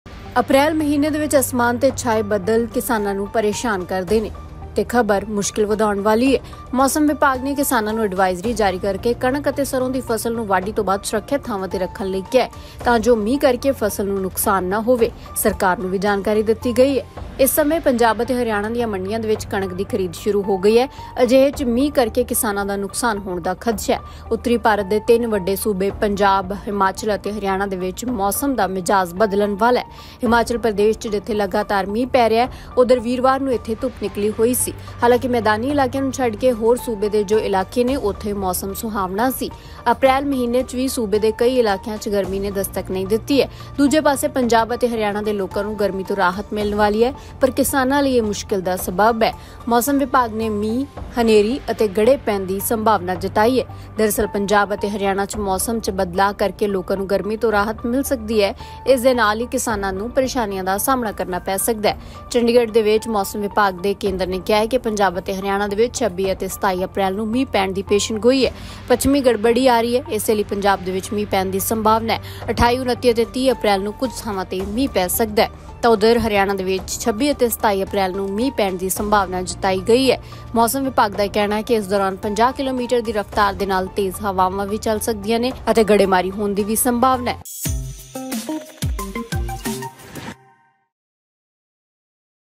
Punjab Weather Update| ਮੁੜ ਪੰਜਾਬ ਦੇ ਕਿਸਾਨਾਂ ਲਈ ਆਫ਼ਤ, ਮੌਸਮ ਵਿਭਾਗ ਦਾ ਅਲਰਟ ਜਾਰੀ
ਮੌਸਮ ਵਿਭਾਗ ਨੇ ਕਿਸਾਨਾਂ ਨੂੰ ਐਡਵਾਈਜ਼ਰੀ ਜਾਰੀ ਕਰਕੇ ਕਣਕ ਅਤੇ ਸਰ੍ਹੋਂ ਦੀ ਫ਼ਸਲ ਨੂੰ ਵਾਢੀ ਤੋਂ ਬਾਅਦ ਸੁਰੱਖਿਅਤ ਥਾਵਾਂ 'ਤੇ ਰੱਖਣ ਲਈ ਕਿਹਾ ਹੈ ਤਾਂ ਜੋ ਮੀਂਹ ਕਾਰਨ ਫ਼ਸਲ ਨੂੰ ਨੁਕਸਾਨ ਨਾ ਹੋਵੇ। ਸਰਕਾਰ ਨੂੰ ਵੀ ਸੂਚਿਤ ਕਰ ਦਿੱਤਾ ਗਿਆ ਹੈ। ਦੱਸ ਦੇਈਏ ਕਿ ਇਸ ਸਮੇਂ ਪੰਜਾਬ ਅਤੇ ਹਰਿਆਣਾ ਦੀਆਂ ਮੰਡੀਆਂ ਵਿੱਚ ਕਣਕ ਦੀ ਖਰੀਦ ਸ਼ੁਰੂ ਹੋ ਗਈ ਹੈ। ਅਜਿਹੇ 'ਚ ਮੀਂਹ ਕਾਰਨ ਕਿਸਾਨਾਂ ਦਾ ਨੁਕਸਾਨ ਹੋਣ ਦਾ ਖਦਸ਼ਾ ਹੈ।
ਉੱਤਰੀ ਭਾਰਤ ਦੇ ਤਿੰਨ ਵੱਡੇ ਰਾਜਾਂ ਪੰਜਾਬ, ਹਰਿਆਣਾ ਅਤੇ ਹਿਮਾਚਲ ਵਿੱਚ ਮੌਸਮ ਦਾ ਪੈਟਰਨ ਬਦਲਣ ਵਾਲਾ ਹੈ। ਹਿਮਾਚਲ ਪ੍ਰਦੇਸ਼ 'ਚ ਜਿੱਥੇ ਲਗਾਤਾਰ ਮੀਂਹ ਪੈ ਰਿਹਾ ਹੈ। ਪਰ ਵੀਰਵਾਰ ਨੂੰ ਇੱਥੇ ਧੁੱਪ ਨਿਕਲੀ। ਹਾਲਾਂਕਿ ਮੈਦਾਨੀ ਇਲਾਕਿਆਂ ਨੂੰ ਛੱਡ ਕੇ ਸੂਬੇ ਦੇ ਹੋਰ ਇਲਾਕਿਆਂ  ਵਿੱਚ ਮੌਸਮ ਸੁਹਾਵਣਾ ਬਣਿਆ ਹੋਇਆ ਹੈ।
ਅਪ੍ਰੈਲ ਮਹੀਨੇ 'ਚ ਵੀ ਸੂਬੇ ਦੇ ਕਈ ਇਲਾਕਿਆਂ 'ਚ ਗਰਮੀ ਨੇ ਦਸਤਕ ਨਹੀਂ ਦਿੱਤੀ। ਦੂਜੇ ਪਾਸੇ ਪੰਜਾਬ ਅਤੇ ਹਰਿਆਣਾ ਵਿੱਚ ਲੋਕਾਂ ਨੂੰ ਗਰਮੀ ਤੋਂ ਰਾਹਤ ਮਿਲਣ ਵਾਲੀ ਹੈ। ਇੱਥੇ ਮੌਸਮ ਵਿਭਾਗ ਨੇ ਮੀਂਹ, ਤੂਫ਼ਾਨ ਅਤੇ ਗੜੇ ਪੈਣ ਦੀ ਸੰਭਾਵਨਾ ਜਤਾਈ ਹੈ।
ਦਰਅਸਲ ਪੰਜਾਬ ਅਤੇ ਹਰਿਆਣਾ 'ਚ ਮੌਸਮ 'ਚ ਬਦਲਾਅ ਕਾਰਨ ਲੋਕਾਂ ਨੂੰ ਗਰਮੀ ਤੋਂ ਰਾਹਤ ਮਿਲੇਗੀ। ਇਸ ਦੇ ਨਾਲ ਹੀ ਕਿਸਾਨਾਂ ਨੂੰ ਪ੍ਰੇਸ਼ਾਨੀਆਂ ਦਾ ਸਾਹਮਣਾ ਕਰਨਾ ਪੈ ਰਿਹਾ ਹੈ। ਚੰਡੀਗੜ੍ਹ ਸਥਿਤ ਮੌਸਮ ਵਿਭਾਗ ਦੇ ਕੇਂਦਰ ਨੇ ਕਿਹਾ ਕਿ ਪੰਜਾਬ ਅਤੇ ਹਰਿਆਣਾ ਵਿੱਚ 26 ਅਤੇ 27 ਅਪ੍ਰੈਲ ਨੂੰ ਮੀਂਹ ਪੈਣ ਦੀ ਸੰਭਾਵਨਾ ਹੈ। ਪੱਛਮੀ ਗੜਬੜੀ ਆ ਰਹੀ ਹੈ ਅਤੇ ਪੰਜਾਬ ਵਿੱਚ ਮੀਂਹ ਪੈਣ ਦੀ ਸੰਭਾਵਨਾ ਹੈ। 28, 29 ਅਤੇ 30 ਅਪ੍ਰੈਲ ਨੂੰ ਕੁਝ ਥਾਵਾਂ 'ਤੇ ਮੀਂਹ ਪੈ ਸਕਦਾ ਹੈ।ਦੂਜੇ ਪਾਸੇ ਹਰਿਆਣਾ ਵਿੱਚ 26 ਅਤੇ 27 ਅਪ੍ਰੈਲ ਨੂੰ ਮੀਂਹ ਪੈਣ ਦੀ ਸੰਭਾਵਨਾ ਹੈ। ਮੌਸਮ ਵਿਭਾਗ ਦਾ ਕਹਿਣਾ ਹੈ ਕਿ ਇਸ ਦੌਰਾਨ 50 ਕਿਲੋਮੀਟਰ ਦੀ ਰਫਤਾਰ ਨਾਲ ਤੇਜ਼ ਹਵਾਵਾਂ ਚੱਲਣਗੀਆਂ ਅਤੇ ਗੜੇਮਾਰੀ ਹੋਣ ਦੀ ਵੀ ਸੰਭਾਵਨਾ ਹੈ।